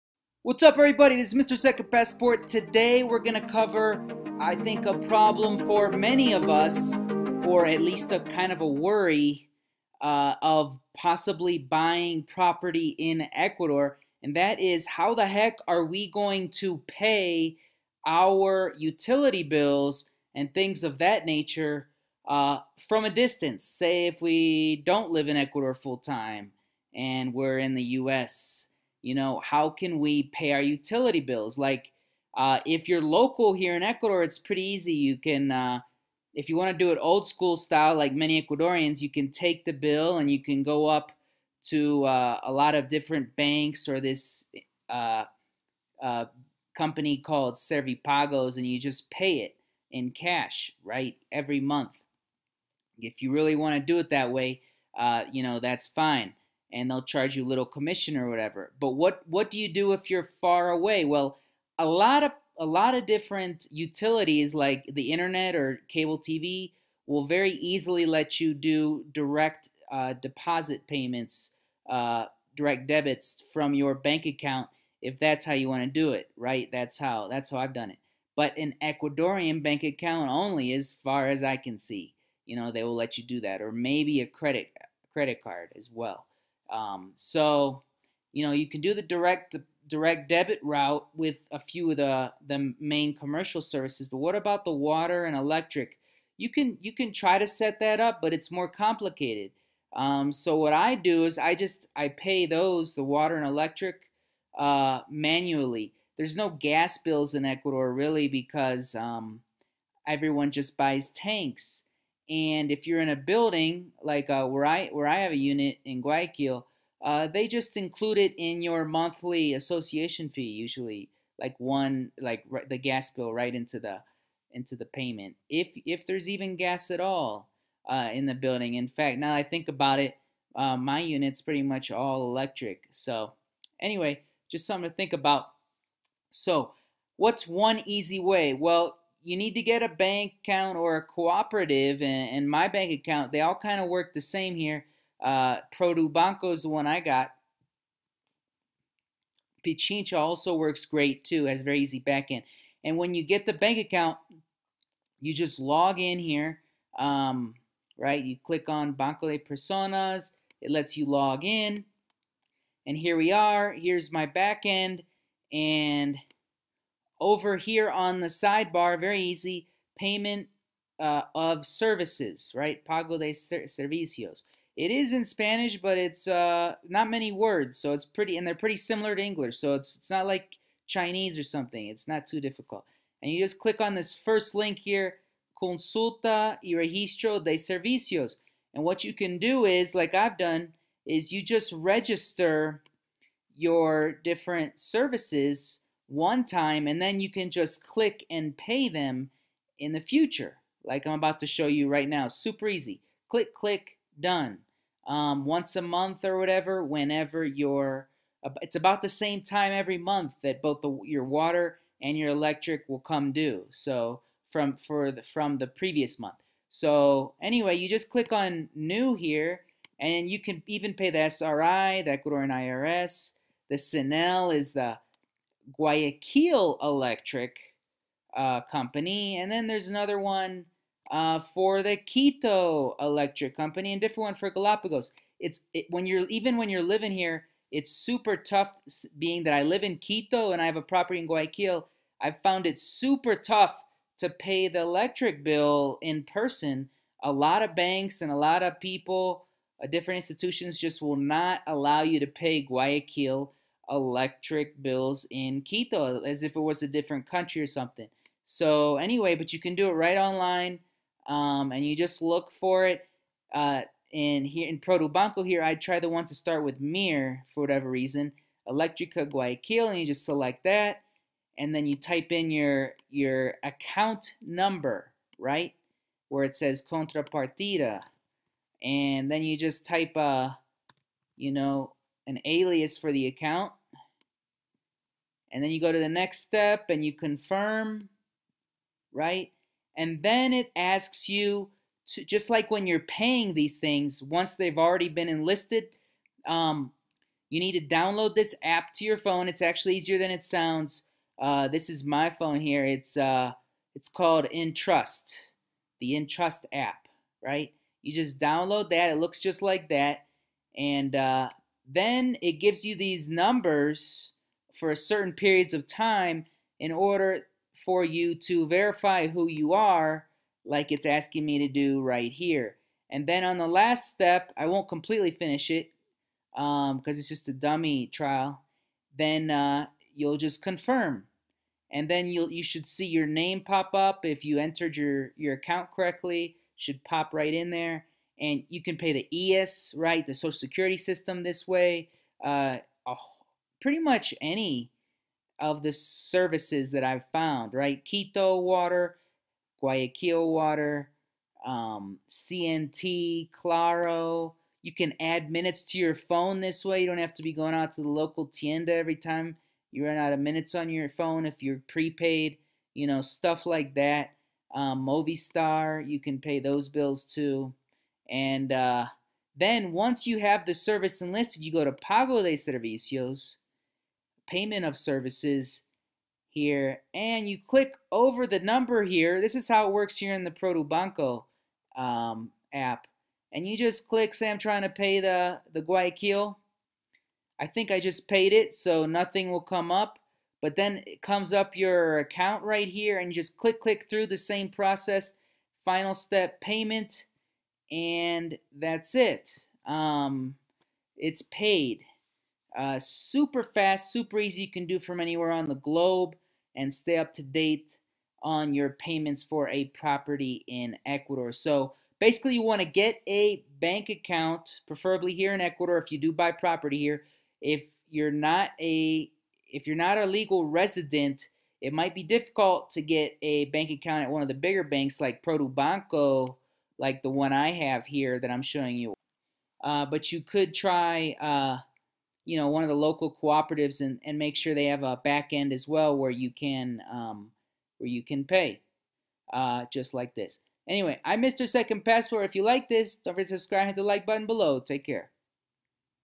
How to pay your utility bills for a PROPERTY in ECUADOR from ANYWHERE
So what happens if you BUY a property in Ecuador, then want to leave it vacant for months out of the year to travel or do other things?

Would you have to hire a manager just to pay a few bills?  Not with what I show you today...

Today in this 5 minute vid Ill show you how I pay my bills when in Ecuador and abroad quick and easy, no exhausting bank lines!...